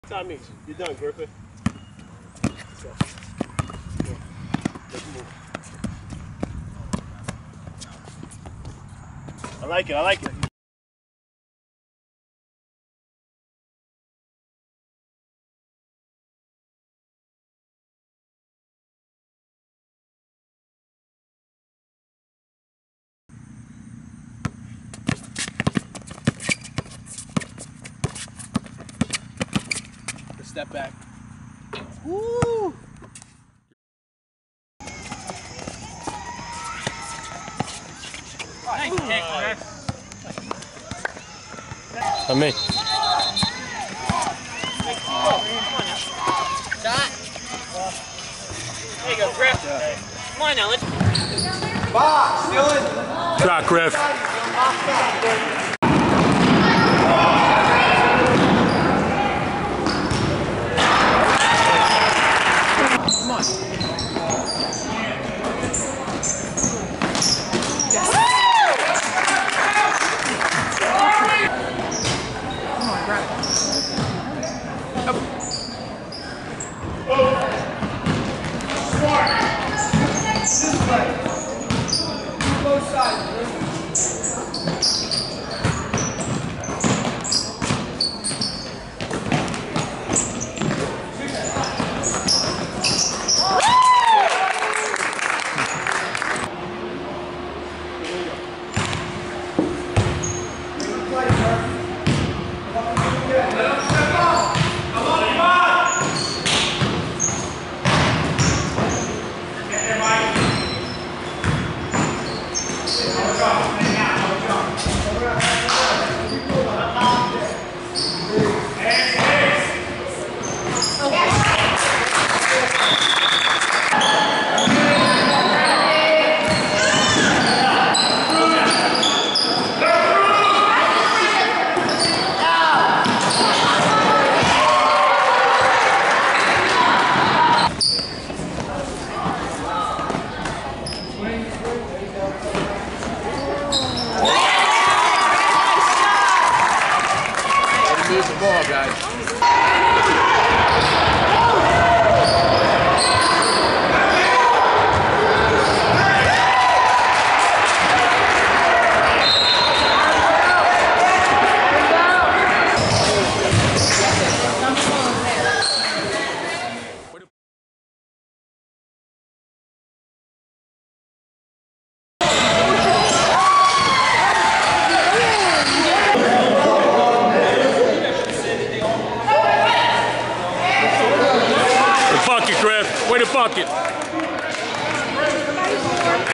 Griffin Murphy Jumpshot Highlights
Griffin Murphy (2017) highlights shooting from working out in August 2011 to playing in winter with different teams.  Featured working out with Mo Bailey & Aaron Morgan.